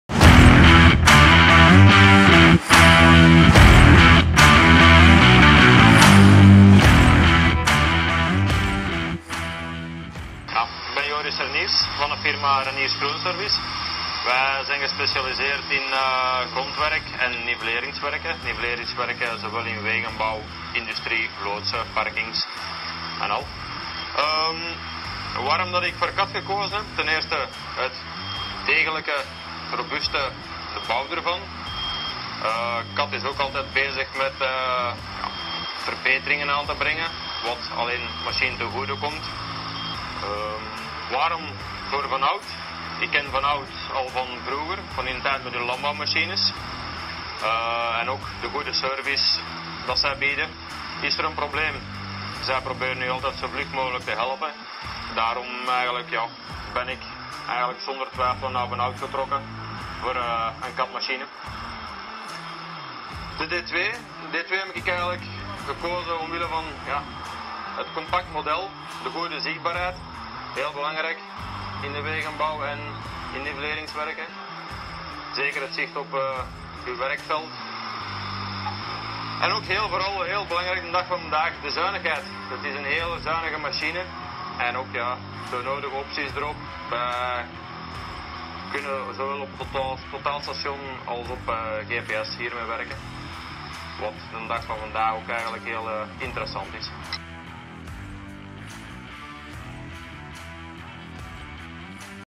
Cat® D2 – Testimonial Joris Reyniers
Nivelleren is een kunst, zeker voor Joris Reyniers ! Dankzij zijn nieuwe dozer, de Cat® D2, beschikt hij over de juiste machine: krachtig, comfortabel, nauwkeurig en compact voor al zijn nivelleerprojecten.
Let’s talk compact!

Niveller est tout un art, surtout pour Joris Reyniers! Grâce à son nouveau bulldozer, la Cat® D2, il possède la machine adéquate : puissante, confortable, précise et compacte pour tous ses projets de nivellement.
Let’s talk compact!